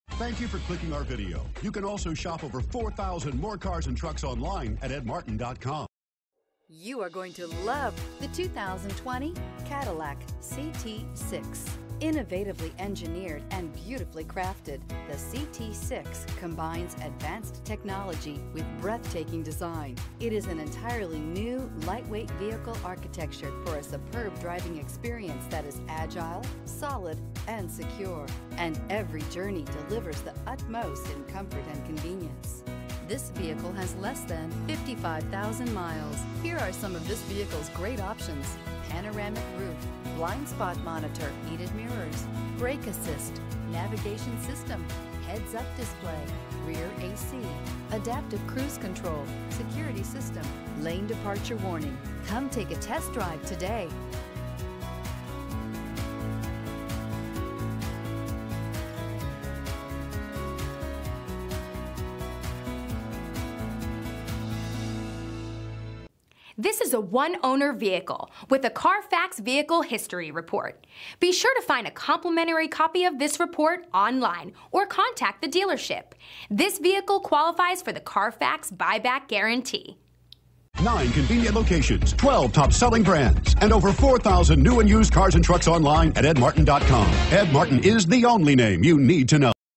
2020 Cadillac CT6 4P3561
2020 CADILLAC CT6 Anderson, IN
Stock# 4P3561

New Price!

Timeless Design: The CT6 boasts a sleek and commanding exterior, combining modern lines with classic Cadillac elegance. It's a true head-turner, from its distinctive grille to its sculpted body.

Powerful Performance: Under the hood, you'll find a range of robust engine options, delivering exhilarating power and a smooth, responsive ride.

Spacious Interior: Step inside the opulent cabin, where premium materials and meticulous craftsmanship create an atmosphere of refinement. The CT6 offers ample space for both passengers and cargo, ensuring comfort and convenience.

Advanced Safety: Cadillac prioritizes safety, equipping the CT6 with an array of advanced safety features to protect you and your passengers.

Experience the epitome of luxury with the 2020 Cadillac CT6. Visit your local dealership today to test drive this remarkable sedan and discover the perfect combination of style, performance, and comfort. Elevate your driving experience with the CT6 and embrace the Cadillac legacy of excellence.

One Owner Trade, BLUETOOTH!, REARVIEW CAMERA!, NAVIGATION, USB PORTS!, Rearview Camera/Backup Camera, Rear DVD System! Dual Screens, Apple Carplay/Android Auto/Carplay, SUPER CRUISE (TM) DRIVER ASSISTANCE FEATURE.

18/27 City/Highway MPG